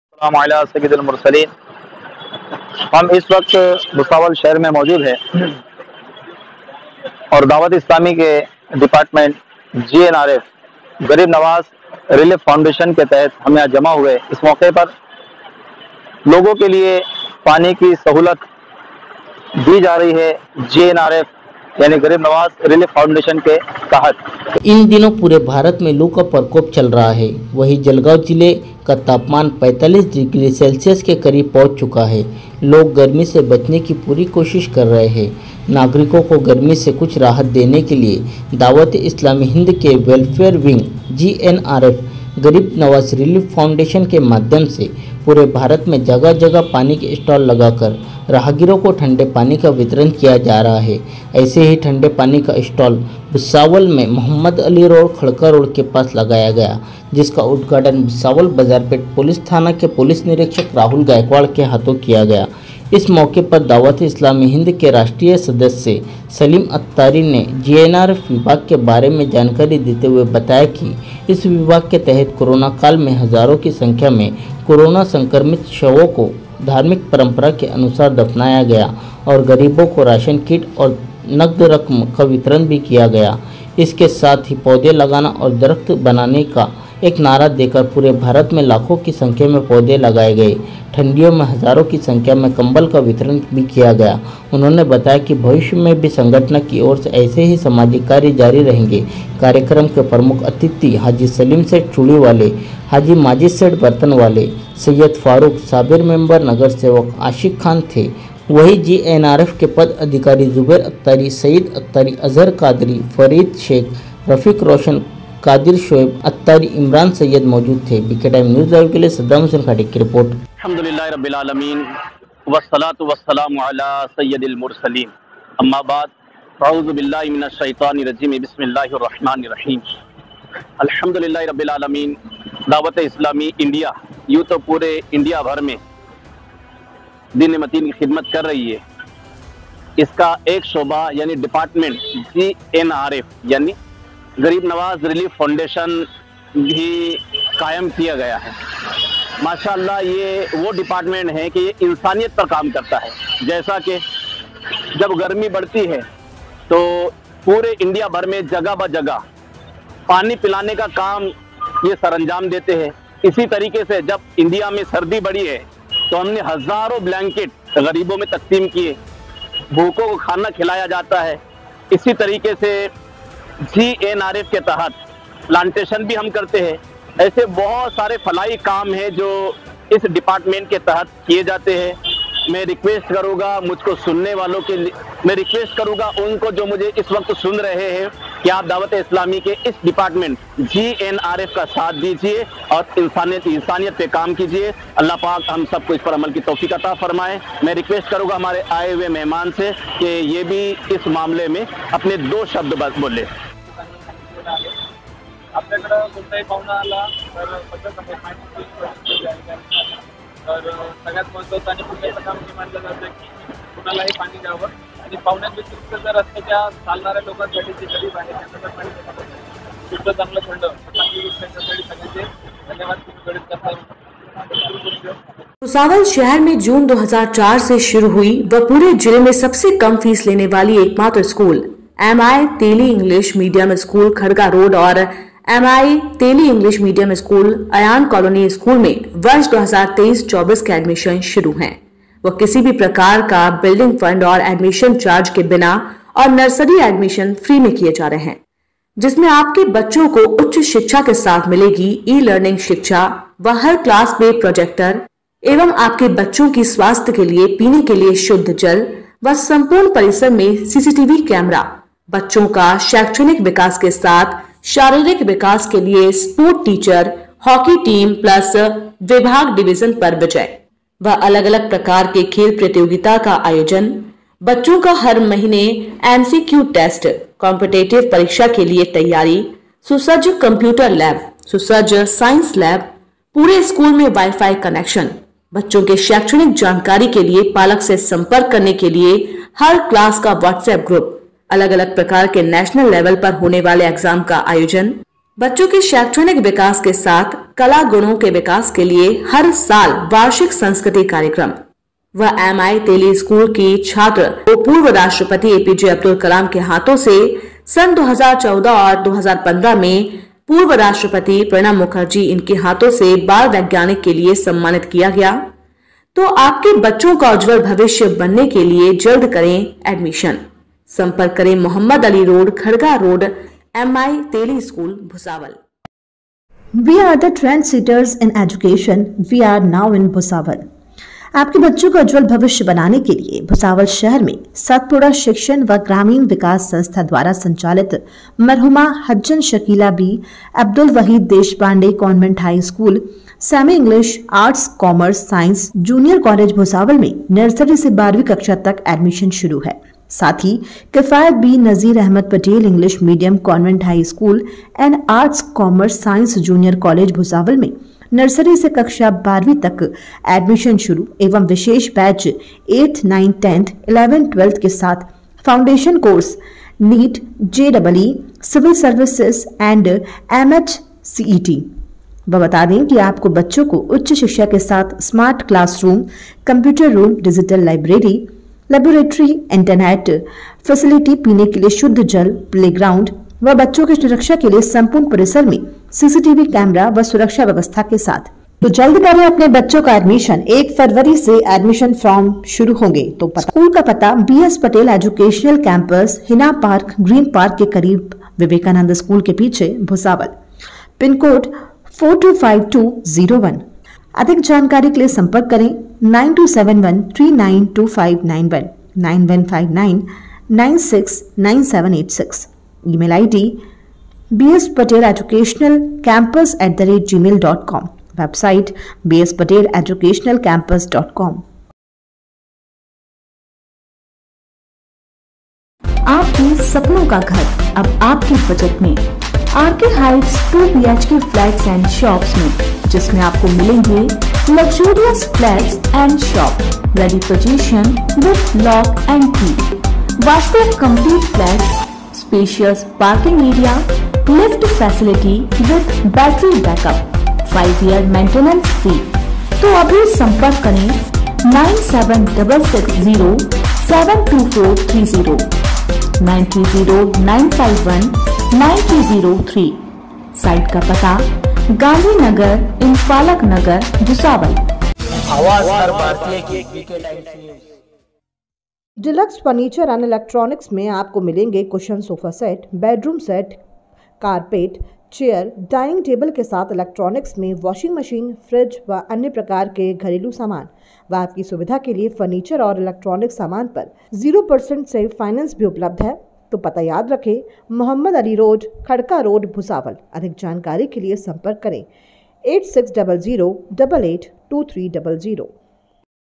भुसावल : में GNRF द्वारा नि:शुल्क ठंडे पानी के स्टॉल का उद्घाटन.! | BK Times न्यूज़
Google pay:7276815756